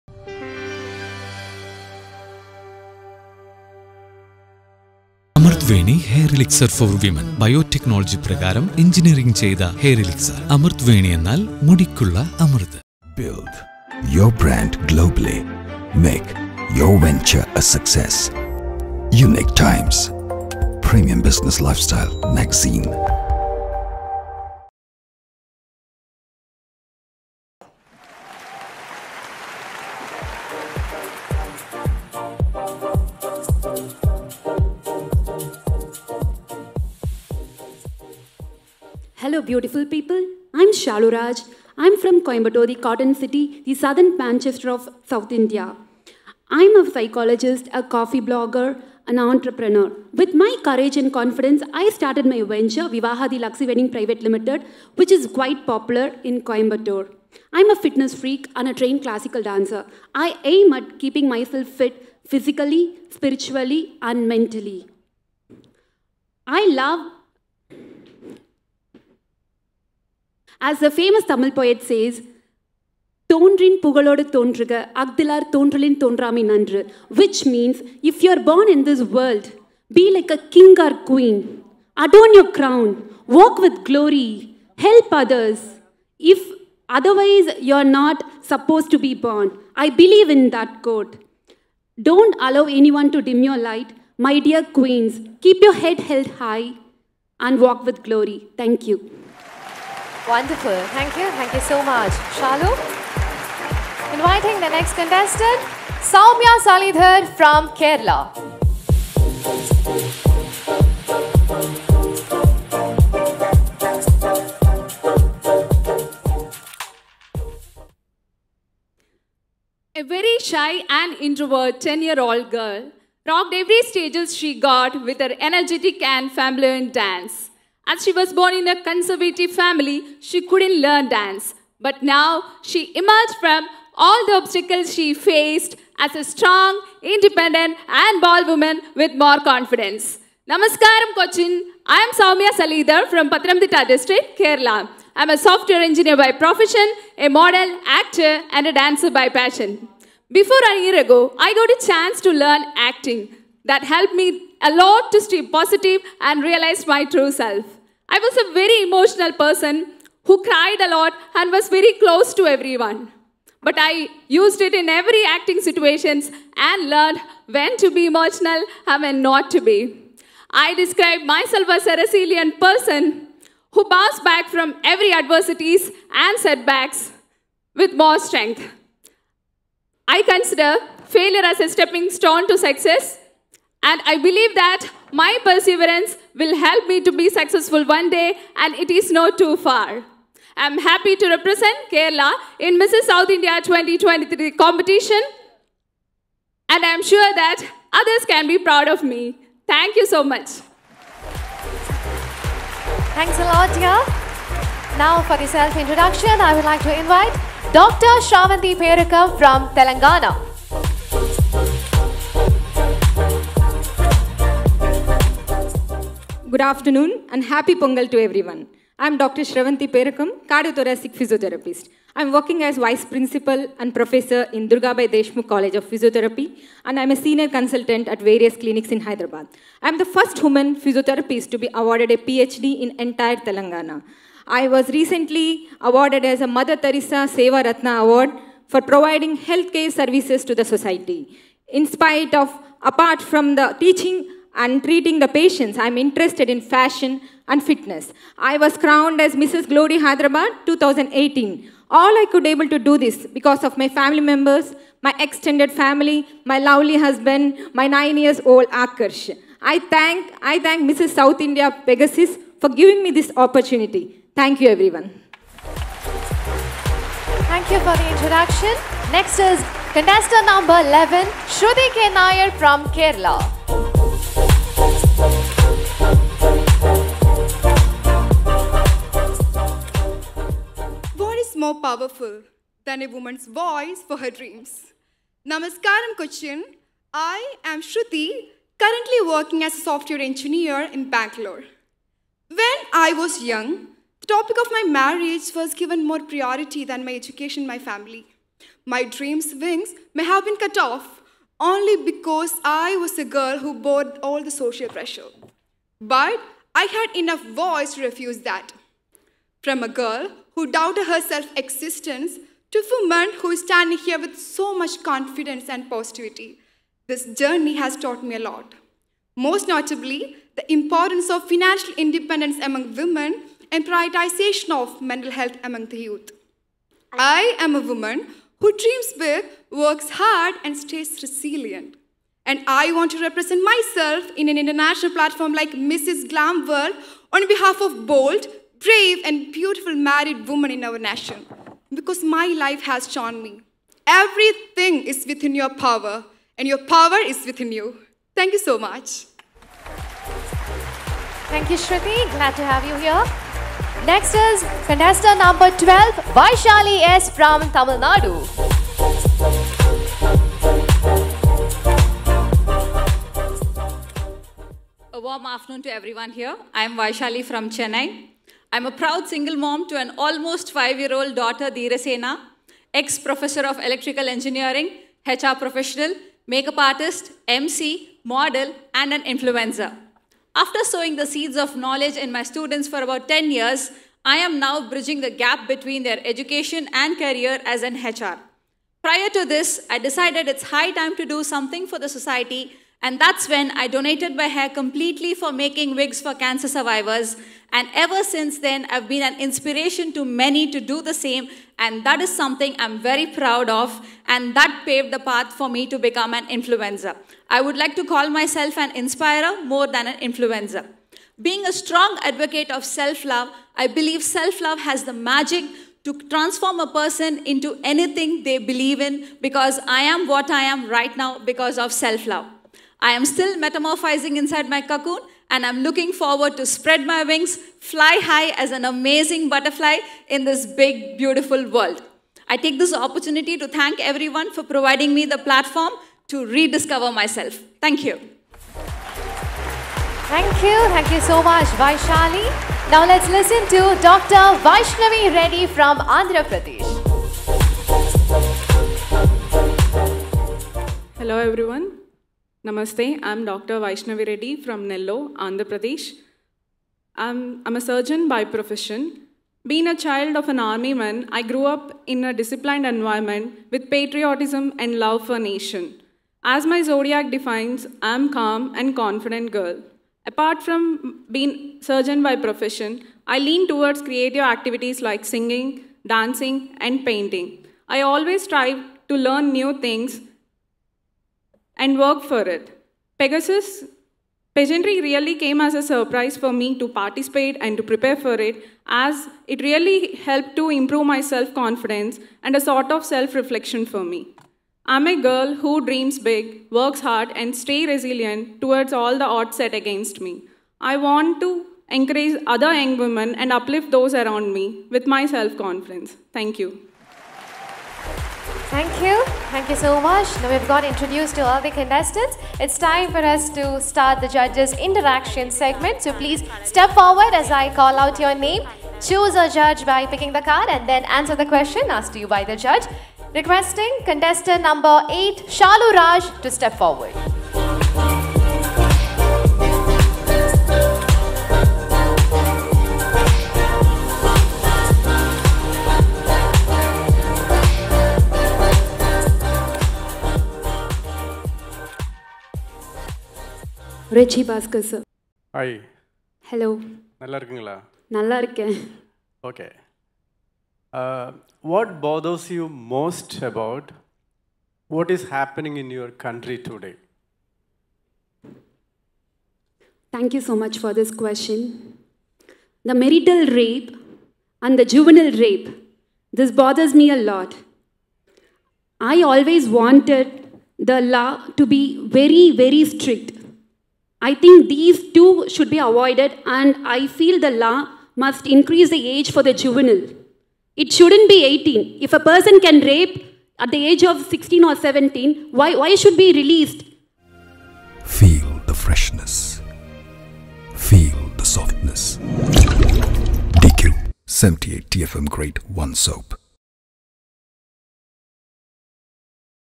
Mrs South India 2023 I Part 5 I An event by Pegasus I Directed by Ajit Ravi Pegasus
MRS SOUTH INDIA 2023

Unique Times and DQUE presents Mrs South India powered by Naturals Salon, Manappuram Finance Limited, Alcazar, Hecate & SAJ Earth Resort - Kochi, Kerala

Winner - Merin John I @merinsarajohn.official
1st runner up - Vaishali S I @vaisha_n_dheera
2nd runner up - Shalu Raj I @vivahabyshalu
Classic Winner - Priyadarsini Deepak I @darshu_darsini

An event by: Pegasus Global
Event directed by: Ajit Ravi